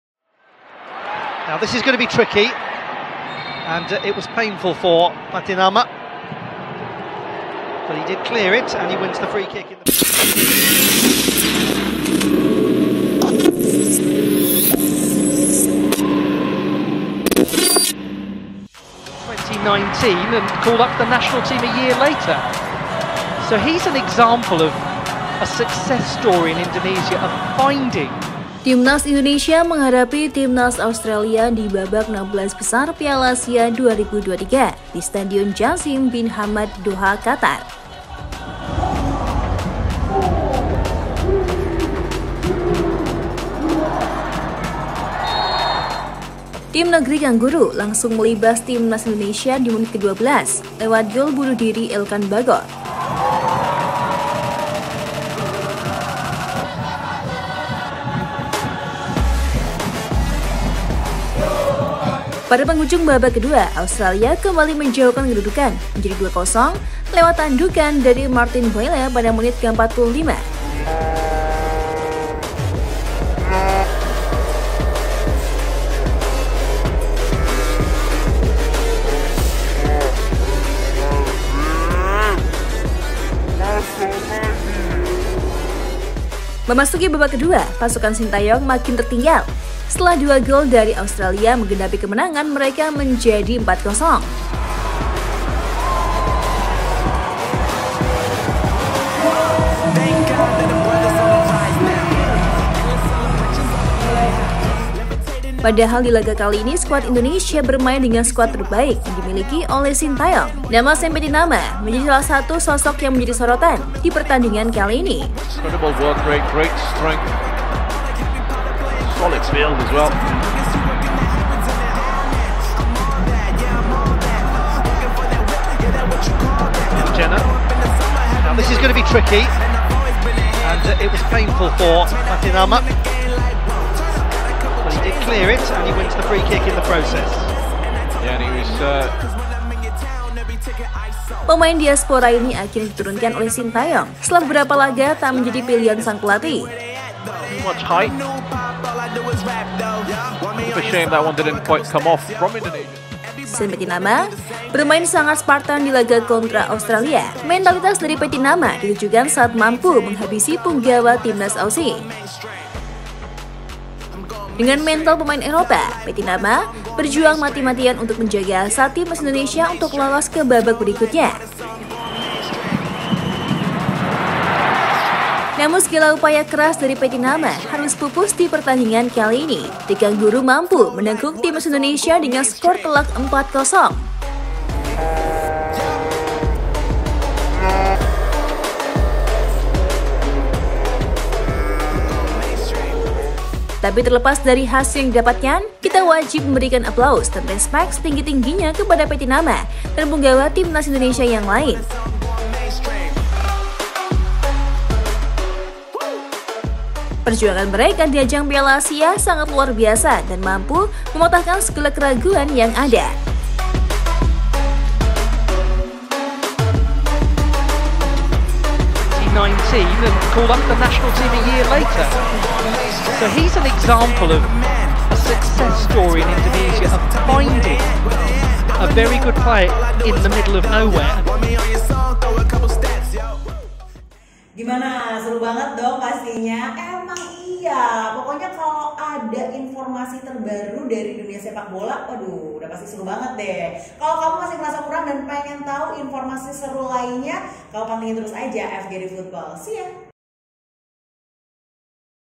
Mentalitas Petarung Pattynama !! Full Individual Skills Pattynama vs Australia 16 Besar Piala Asia
Mentalitas Petarung Pattynama  !! Full Individual Skills Shayne Pattynama vs Australia di 16 Besar Piala Asia